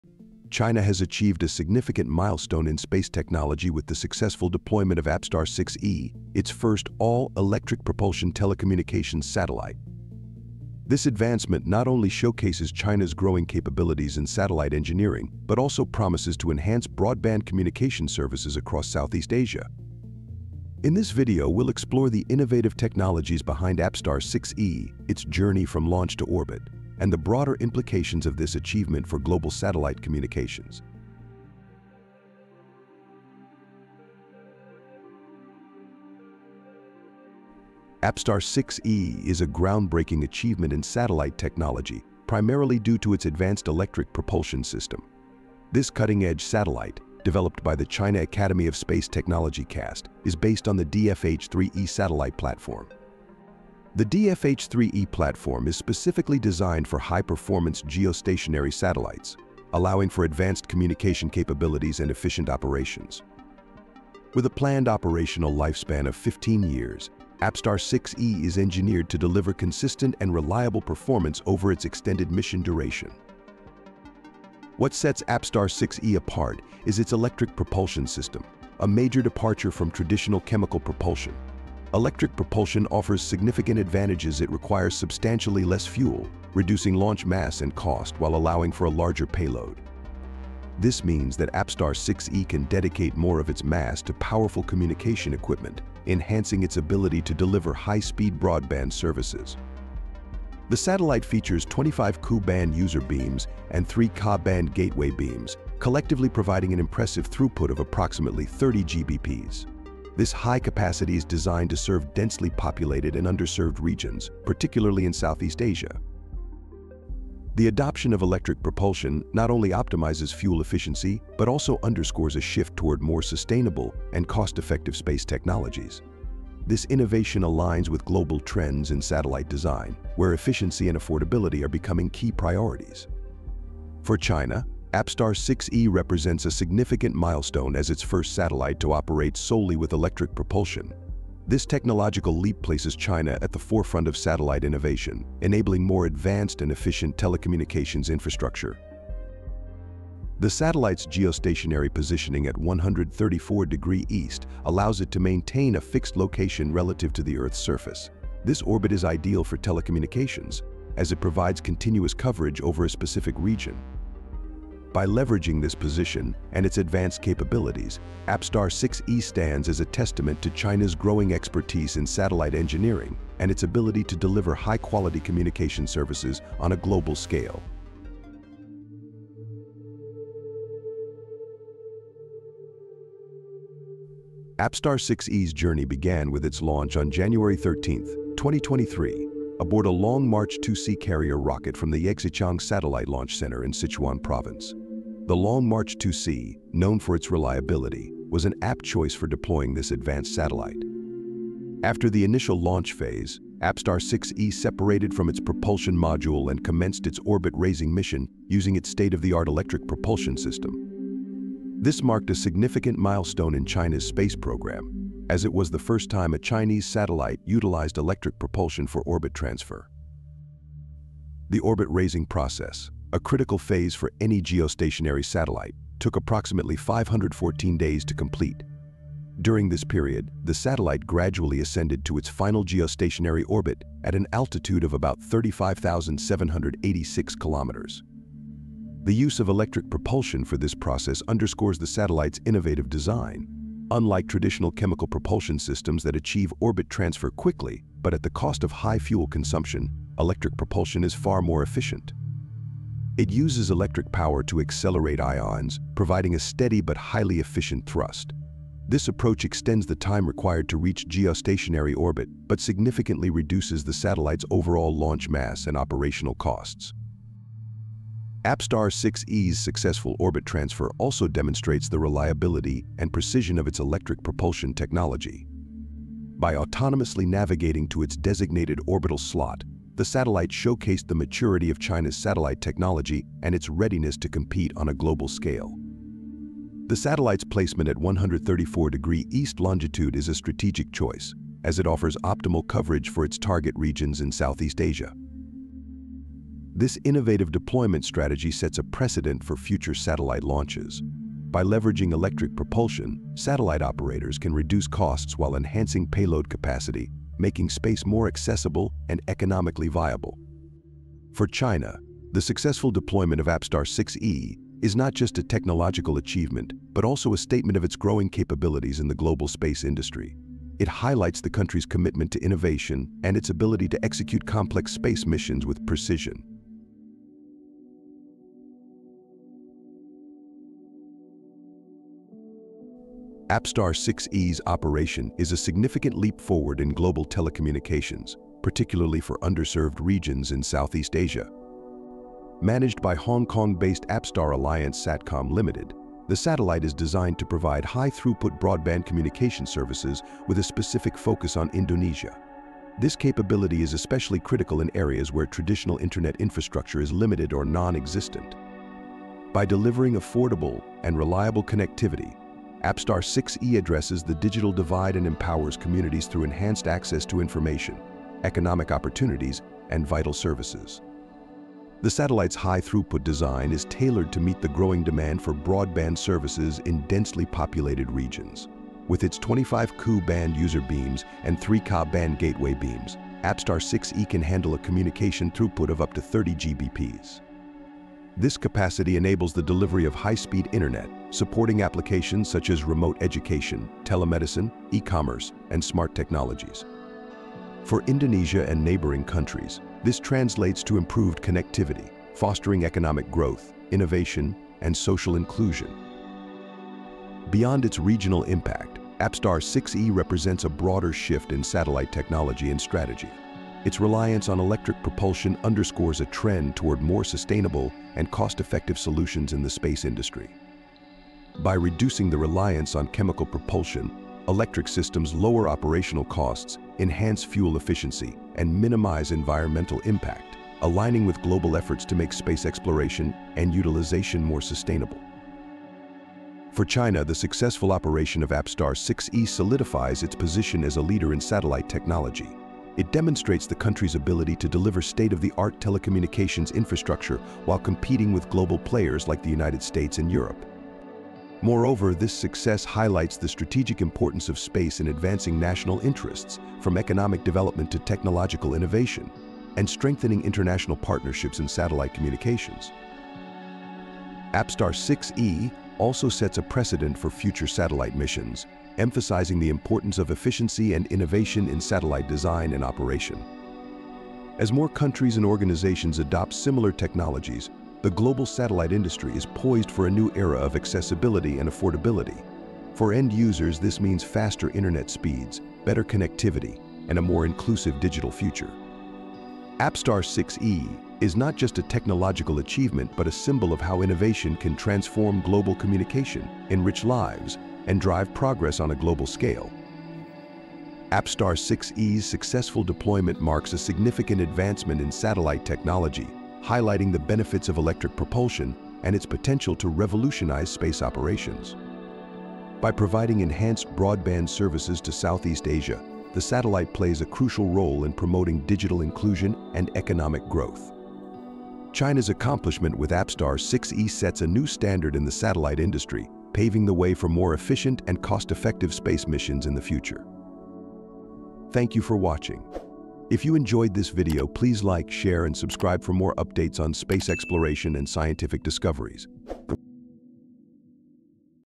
Revolution: China just delivered APSTAR-6E, its first all-electric satellite, into orbit!
China’s APSTAR-6E, the first all-electric propulsion telecommunications satellite, marks a revolutionary step in space technology. Developed on the advanced DFH-3E platform, this satellite uses cutting-edge electric propulsion to enhance fuel efficiency, reduce launch costs, and maximize communication capabilities. Launched in 2023 via a Long March-2C rocket, it now delivers high-speed, affordable broadband to underserved regions like Indonesia. This milestone highlights China’s growing leadership in satellite innovation and its role in bridging the digital divide. APSTAR-6E’s breakthrough technology and 15-year lifespan set a new standard for sustainable, efficient satellite operations. Explore how this satellite is transforming global connectivity and paving the way for future advancements in space communications.